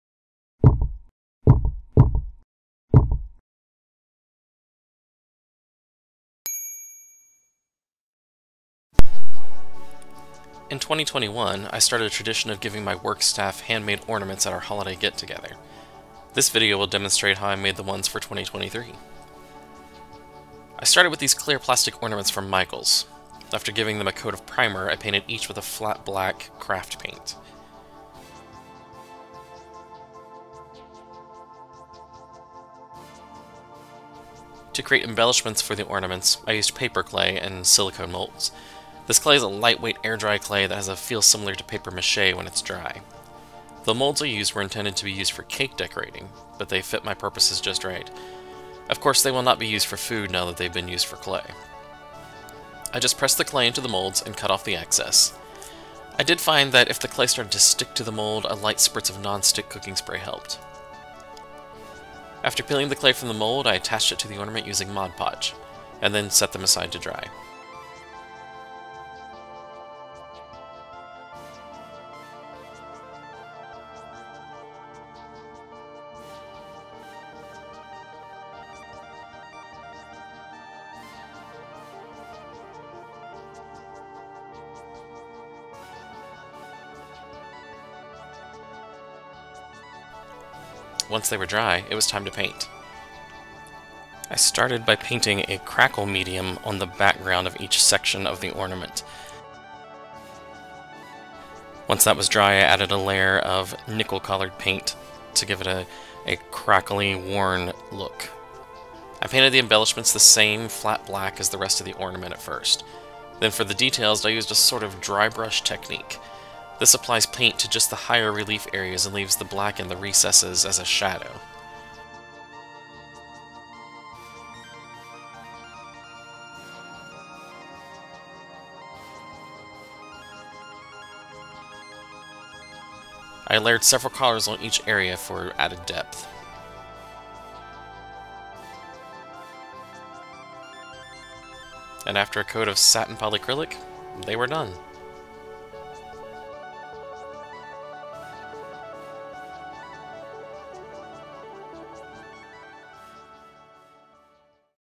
Hand Embellished Christmas Ornaments
Tutorial on how I made gift ornaments using paper clay and Mod Podge.